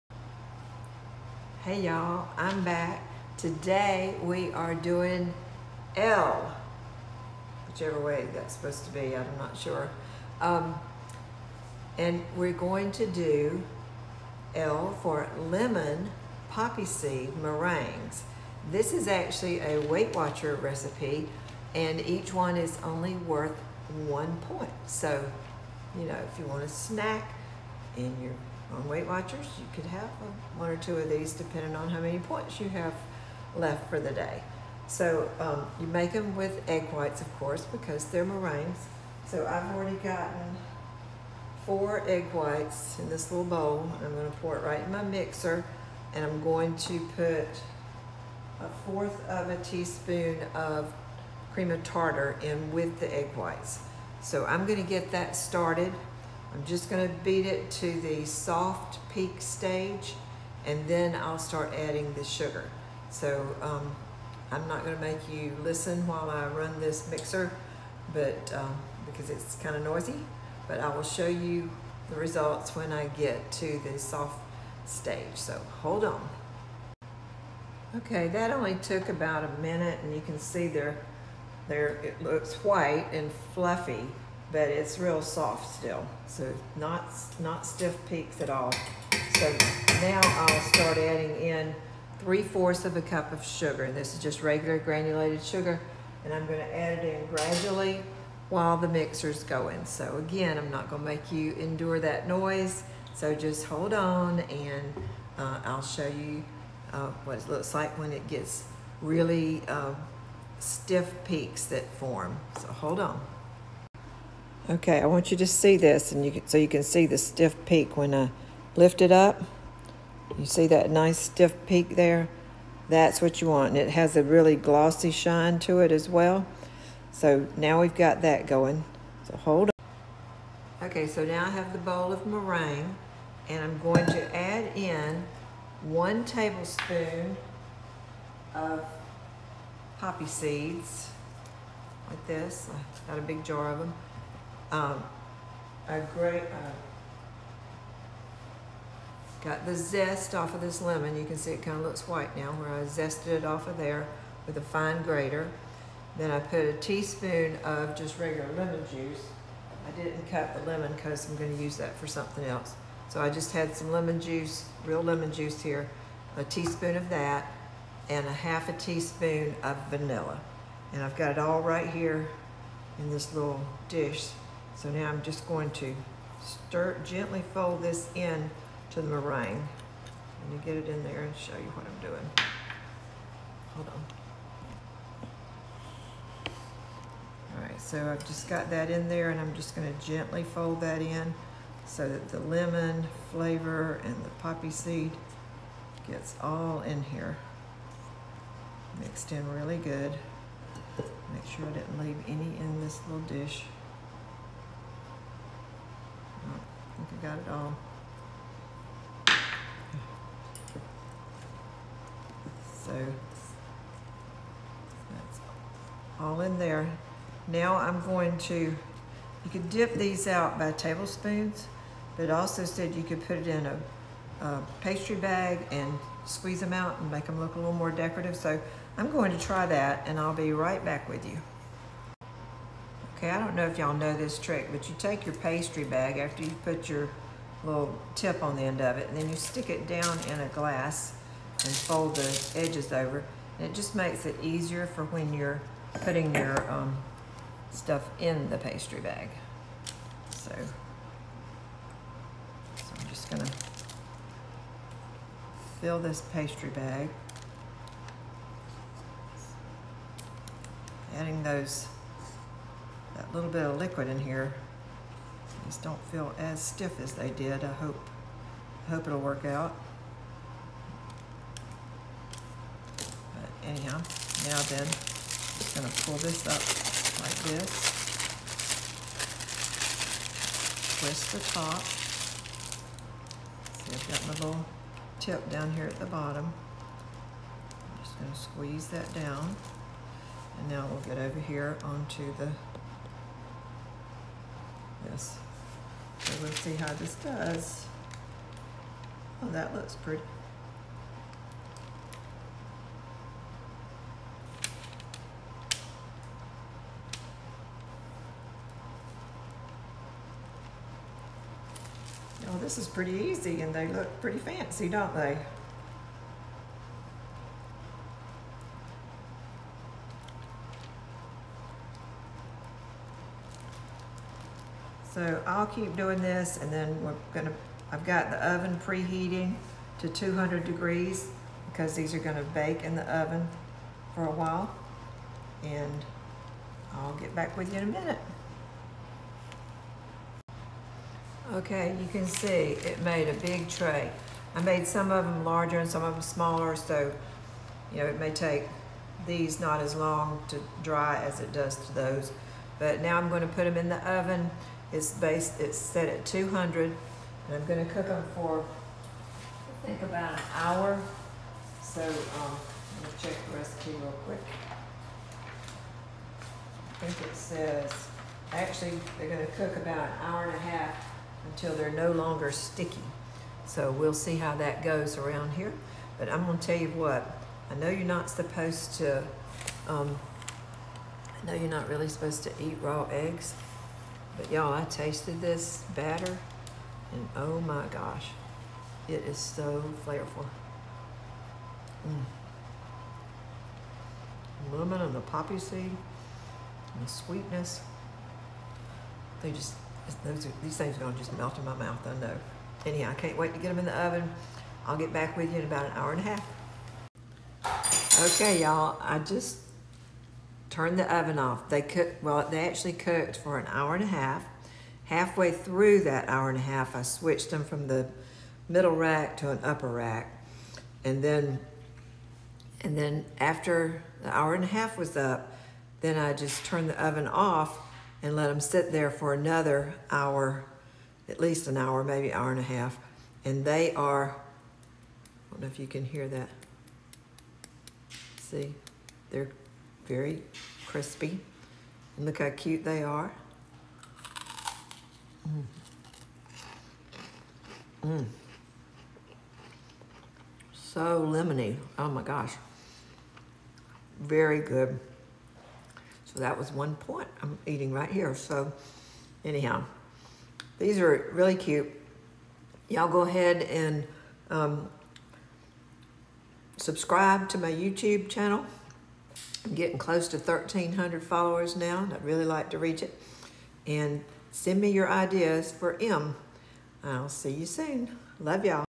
L is for Lemon Poppyseed Meringues
L is for Lemon Poppyseed meringues! So delicious and only 1 point each on Weight Watchers!